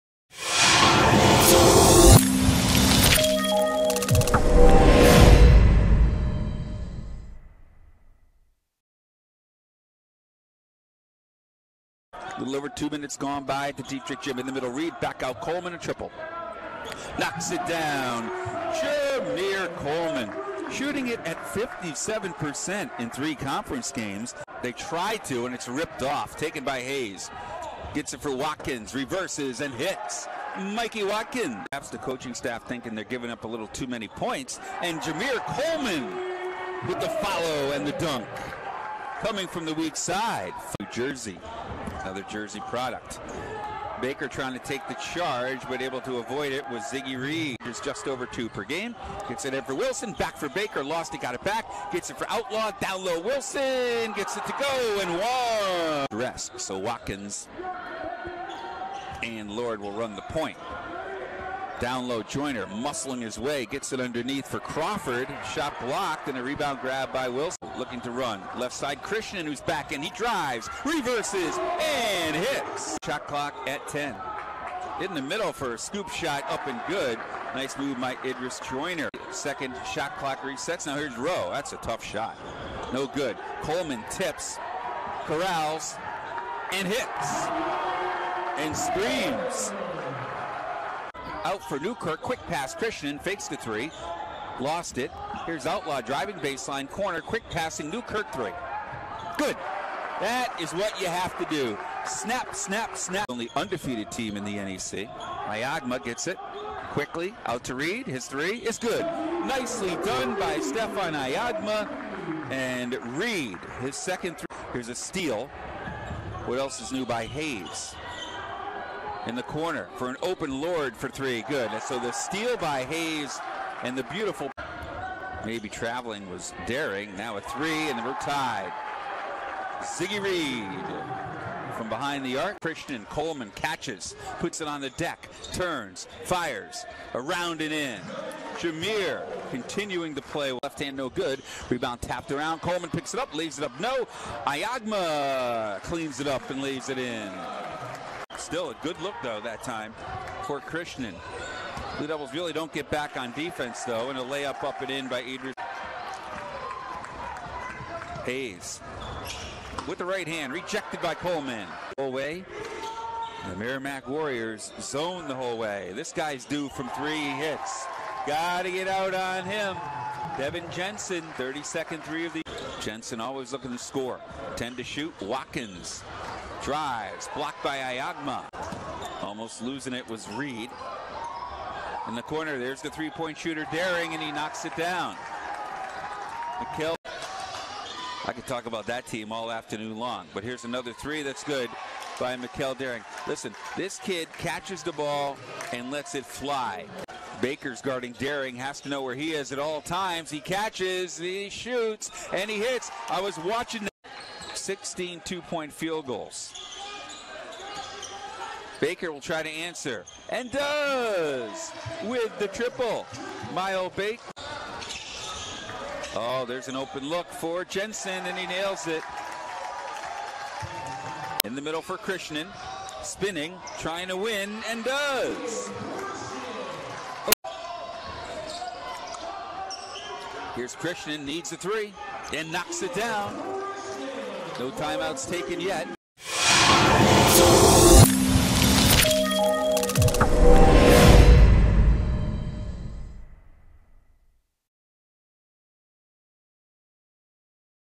Men's Basketball Highlights | CCSU vs. Merrimack 01/11/20
Highlights of the Northeast Conference men's basketball game between Central Connecticut State University and Merrimack College on Jan. 11, 2020 at Detrick Gymnasium in New Britain, CT